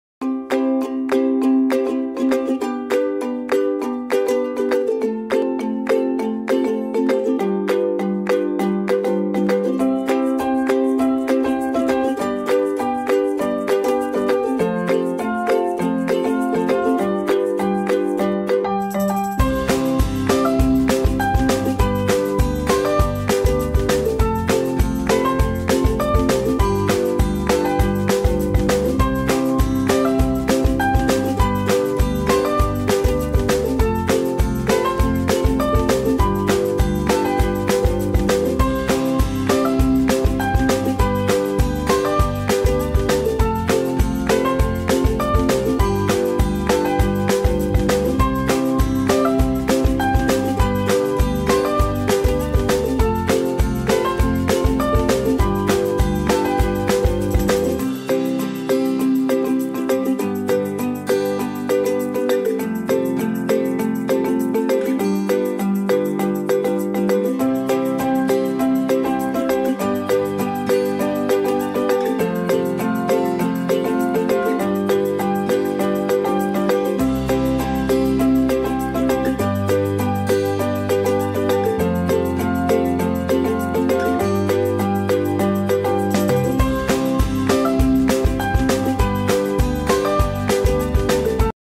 PPI SHIZUOKA 2016 - 2017
Seklias Struktur Organsisasi dan Program Kerja PPIJ Komsat Shizuoka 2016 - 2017